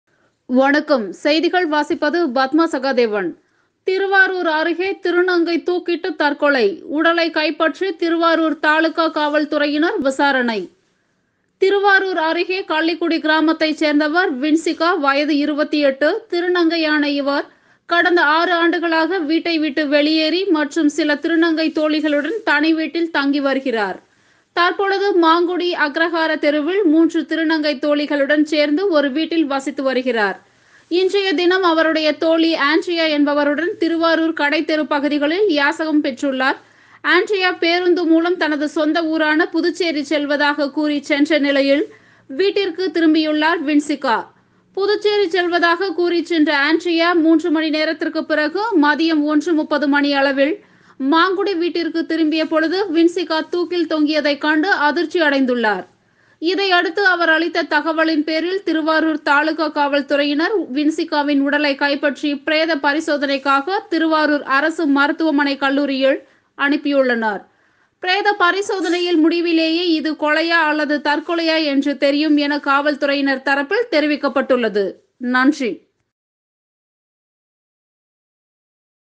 திருவாரூர் அருகே திருநங்கை தூக்கிட்டு தற்கொலைஉடலை கைப்பற்றி திருவாரூர் தாலுகா காவல்துறையினர் விசாரணை
திருவாரூர்.                                   25.11.21

திருவாரூர் அருகே கள்ளிக்குடி கிராமத்தைச் சேர்ந்தவர் வின்சிகா(28) திருநங்கையான இவர் கடந்த 6 ஆண்டுகளாக வீட்டை விட்டு வெளியேறி மற்றும் சில திருநங்கை தோழிகளுடன் தனி வீட்டில் தங்கி வருகிறார். தற்பொழுது மாங்குடி அக்ரகார தெருவில் 3 திருநங்கை தோழிகளுடன் சேர்ந்து ஒரு வீட்டில் வசித்து வருகிறார்.

இன்றைய தினம் அவருடைய தோழி ஆண்ட்ரியா என்பவருடன் திருவாரூர் கடைத்தெரு பகுதிகளில் யாசகம் பெற்றுள்ளார். ஆண்ட்ரியா பேருந்து மூலம் தனது சொந்த ஊரான புதுச்சேரி செல்வதாக கூறி சென்ற நிலையில் வீட்டிற்கு திரும்பியுள்ளார் வின்சிகா. புதுச்சேரி செல்வதாக கூறி சென்ற ஆண்ட்ரியா 3 மணி நேரத்திற்கு பிறகு மதியம் ஒன்று முப்பது மணி அளவில் மாங்குடி வீட்டிற்கு திரும்பிய பொழுது
வின்சிகா தூக்கில் தொங்கியதை கண்டு அதிர்ச்சி அடைந்துள்ளார்.

 இதையடுத்து அவர் அளித்த தகவலின் பேரில் திருவாரூர் தாலுகா காவல் துறையினர் வின்சிகாவின் உடலை கைப்பற்றி பிரேத பரிசோதனைக்காக திருவாரூர் அரசு மருத்துவக்கல்லூரி மருத்துவமனைக்கு அனுப்பியுள்ளனர். பிரேத பரிசோதனையில் முடிவிலேயே இது கொலையா அல்லது தற்கொலையா என்ற தெரியும் என காவல்துறையினர் தரப்பில் தெரிவிக்கப்பட்டுள்ளது.